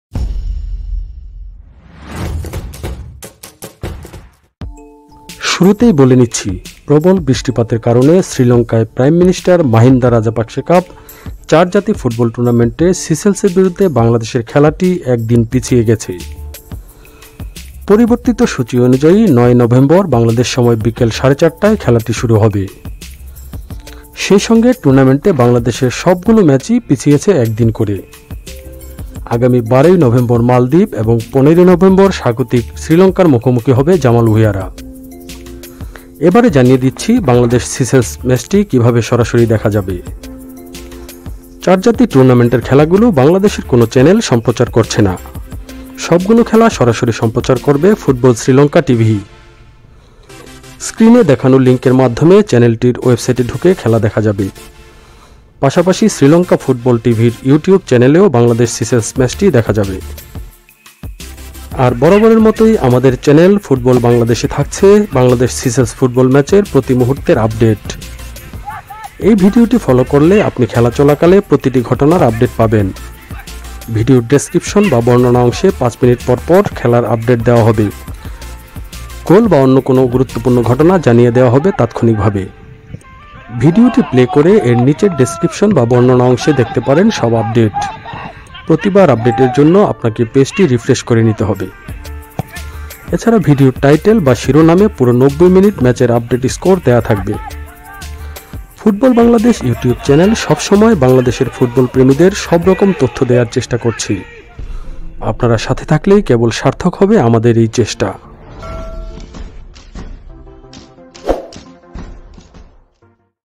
Bangladesh vs Seychelles Football । Live Update । বাংলাদেশ বনাম সিশেলস ম্যাচ লাইভ দেখবেন কীভাবে
আপডেট তথ্য
অতিরিক্ত বৃষ্টিপাতের কারণে শ্রীলঙ্কায় চার জাতি টুর্নামেন্টে সিশেলসের বিরুদ্ধে বাংলাদেশ জাতীয় ফুটবল দলের ম্যাাচটি আবারো পিছিয়ে গেলো। আজ বিকেল সাড়ে চারটায় নির্ধারিত সময়ে ম্যাচটি হচ্ছে না। দুই দলের মধ্যকার খেলাটি কবে কখন অনুষ্ঠিত হবে তা এখনো নির্ধারণ করেনি ফুটবল শ্রীলঙ্কা। ঘোষণা এলেই এখানে জানিয়ে দেয়া হবে।

Bangladesh vs Seychelles Football । Live Update ।
How to Watch Live Bangladesh Seychelles Football Match
বাংলাদেশ বনাম সিশেলস ফুটবল ম্যাচ লাইভ দেখবেন কীভাবে